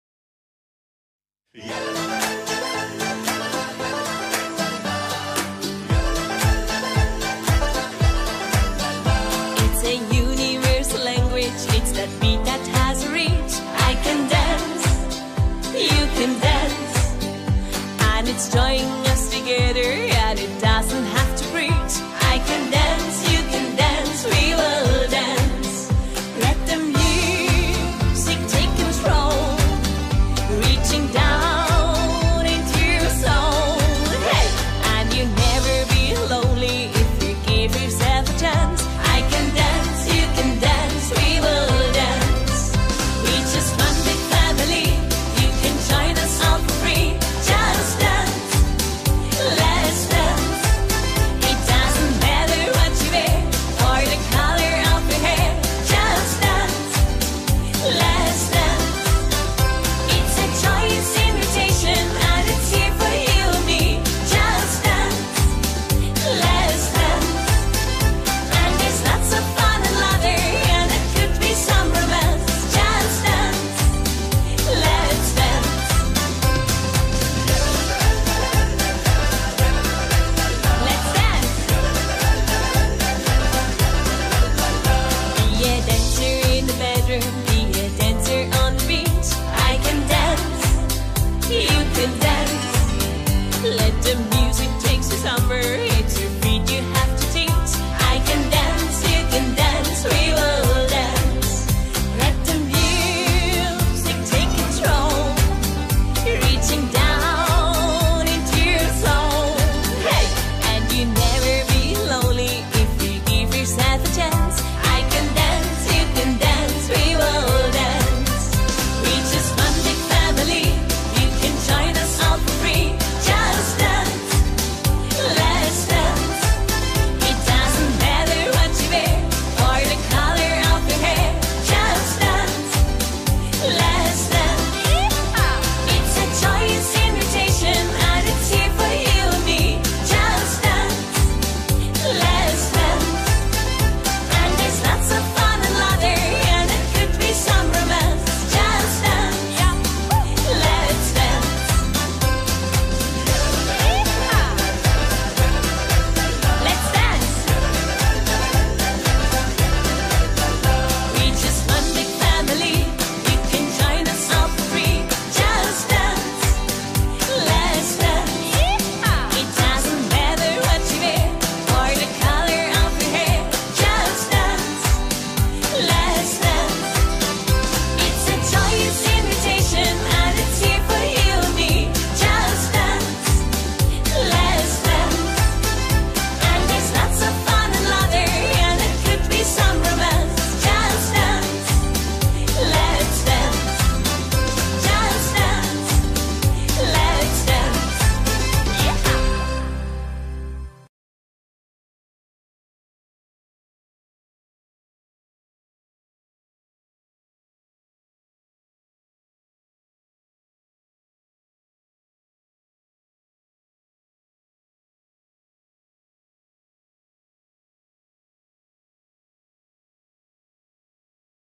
Constance Rock - Line Dance/ Improver
32 count, 4 Wall, 2 Restarts, Improver Level, Choreo by Gudrun Schneider, Ivonne  Verhagen , Remco Zwijgers, Fabian Muller, Marianne Fuhrt & Marion Hofman  Demo by Daisy & Friends